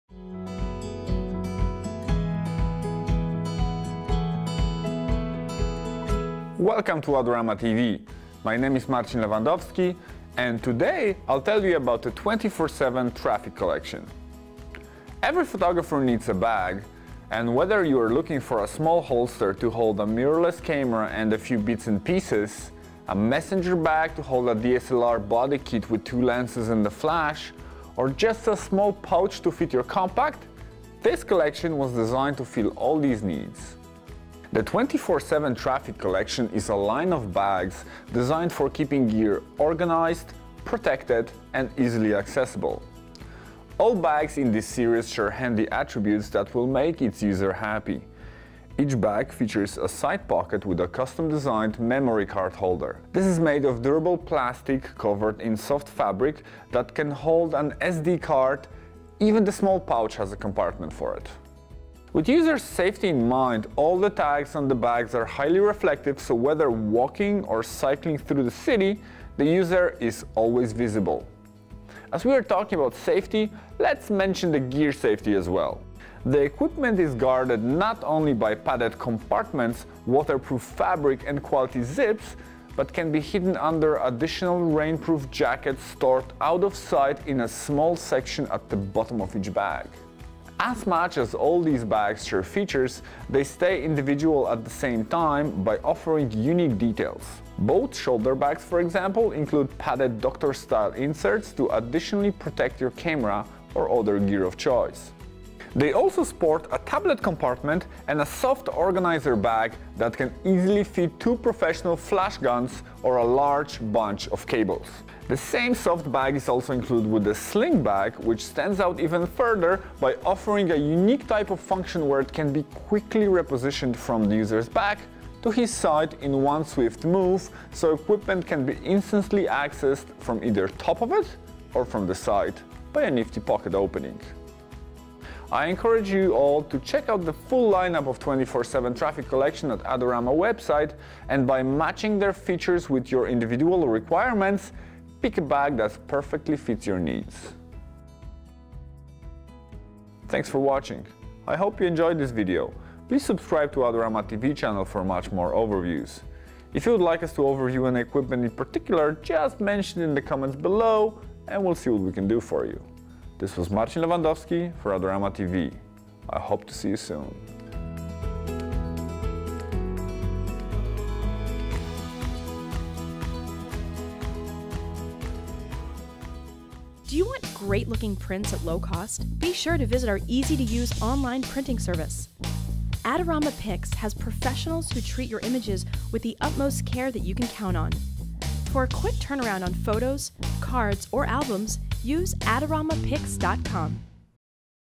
Traffic 24/7 Bag Collection : Product Overview with Marcin Lewandowski
In this product overview video, Marcin looks at the various bags that are a part of the 24/7 Traffic Collection.

Photographs by Marcin Lewandowski